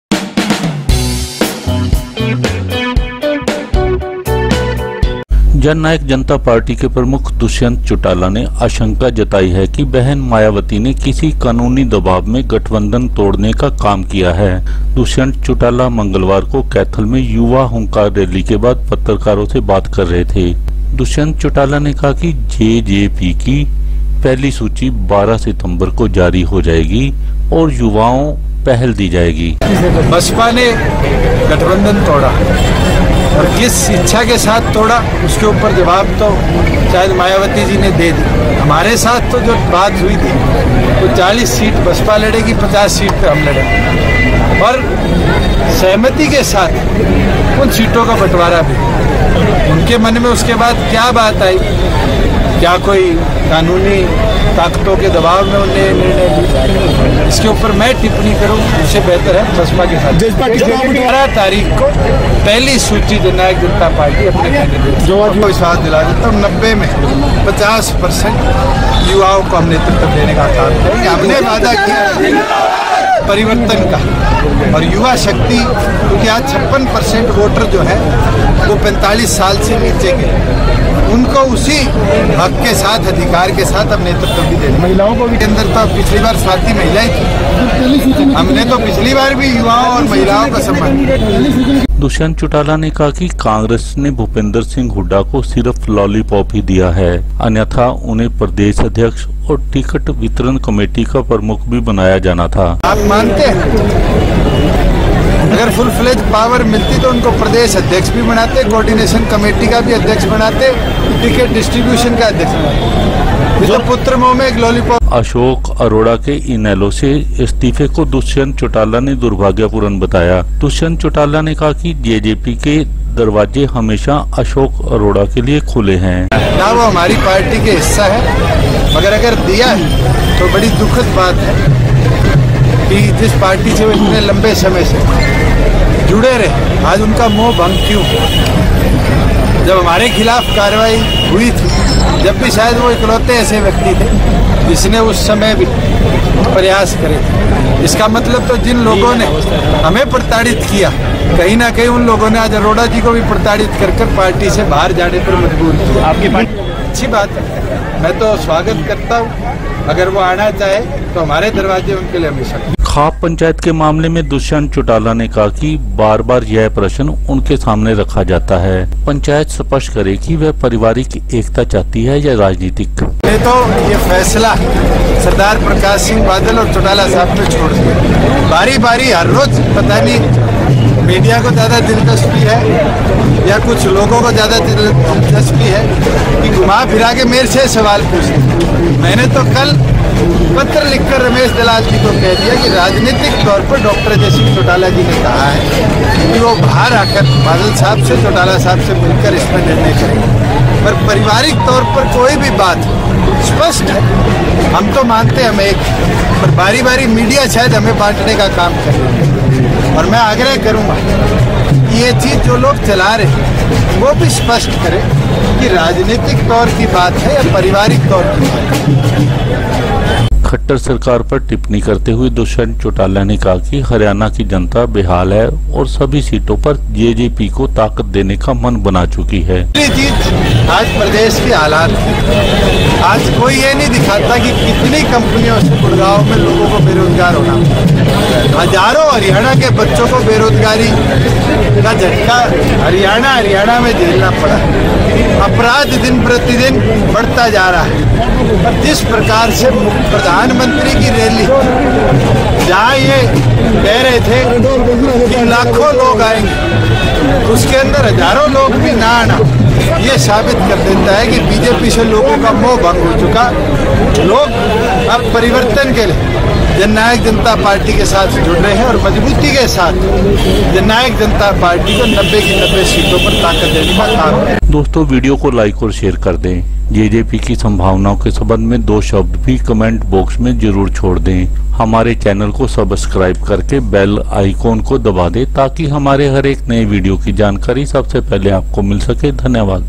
Dushyant Chautala को आशंका क़ानूनी दबाव में Mayawati ने तोडा गठबंधन
This video is about Jannayak Janta Party chief Dushyant Chautala visit to Kaithal. After participation in Yuva Hunkar rally Kaithal, Dushyant Chautala spoke to media. Dushyant Chautala said that BSP chief Mayawati may be under pressure while breaking ties with JJP. Dushyant Chautala said that JJP will contest all 90 seats in Haryana assembly election 2019 and women and youth will be preferred while distributing tickets. Dushyant Chautala said that first list of JJP candidates will be out on 12th September. Reaction to Bhupinder Singh Hooda elevation as CLP, Dushyant Chautala said he has merely given lollipop by congress high command. Dushyant Chautala termed resignation of Ashok Arora from INLD as unfortunate. Dushyant Chautala said that doors of JJP will always be opened for Ashok Arora. Dushyant Chautala said that voters of Haryana want to get rid of BJP government and leaning towards JJP.